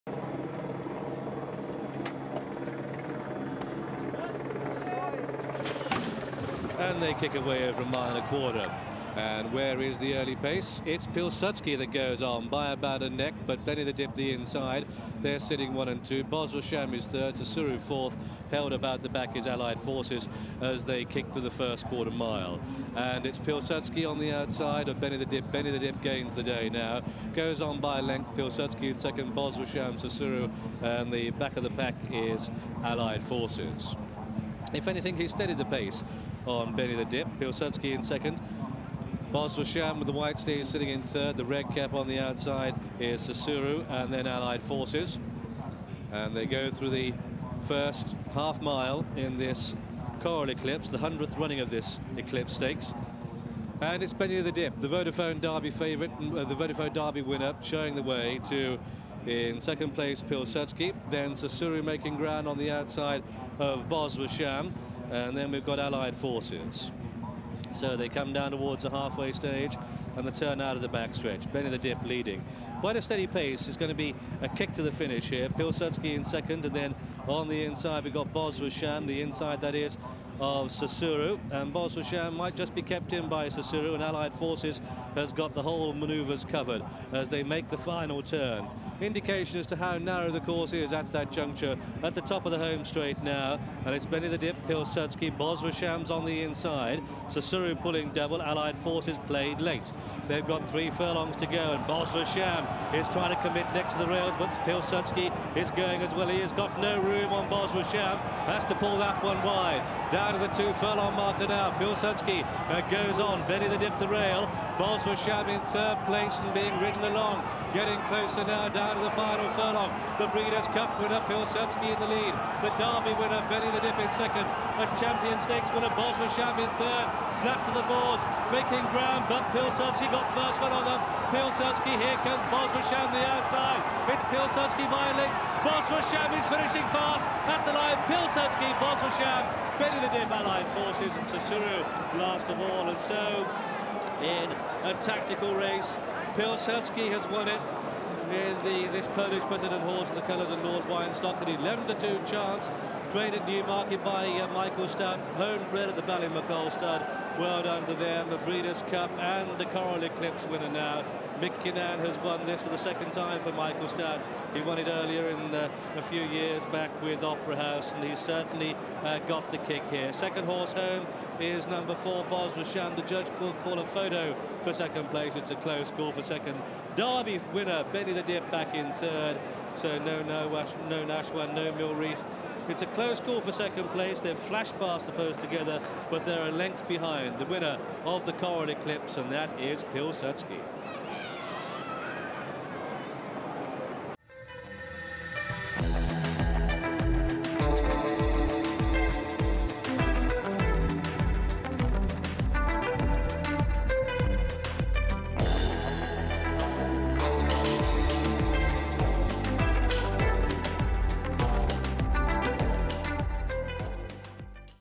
No luck for Bosra Sham as PILSUDSKI beats Derby winner Benny The Dip in the 1997 Coral-Eclipse
Pilsudski was the highest-ranked older horse of 1997 in the International Classifications with a rating of 134, supplementing success in this race, the Irish Champion Stakes, Champion Stakes and Japan Cup with second places in the King George and the Arc de Triomphe.

Meanwhile, 20 years on from this race in 2017, jockey Kieren Fallon admitted that he still thinks about the luckless run endured by Bosra Sham.